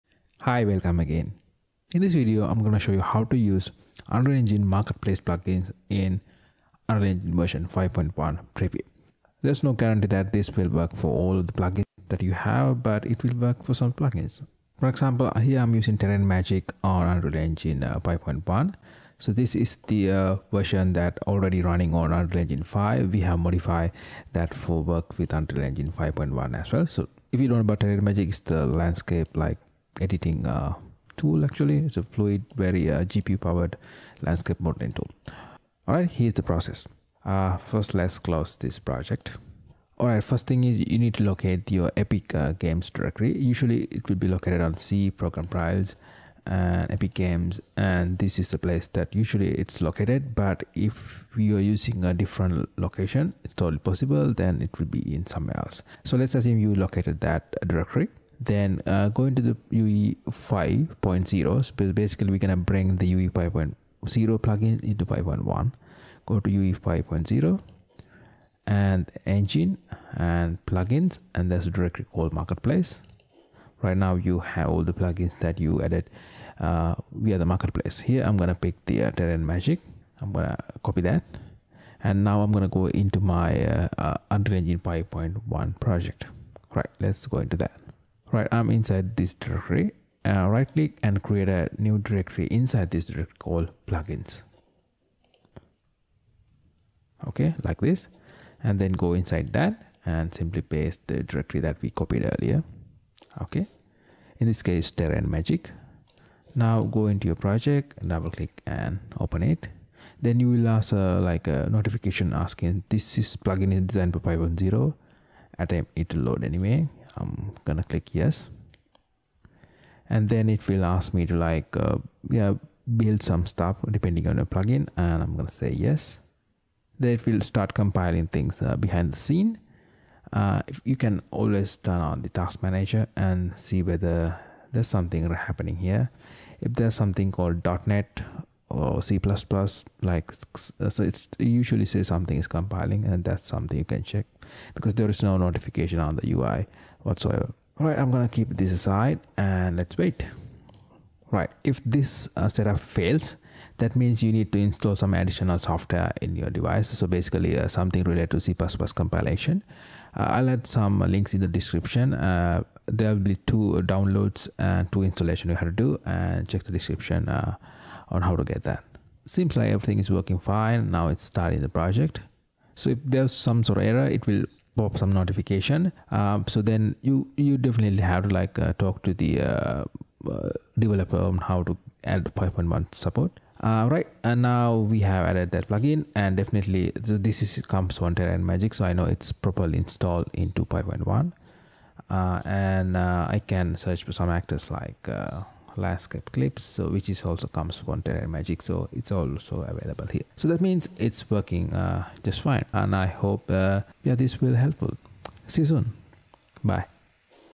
How to Install Marketplace Plugins on Unreal Engine 5.1
In this video, I am showing you how to install Unreal Engine Marketplace plugins on UE 5.1. If you have not set up your PC for C++ development, try to install the following tools.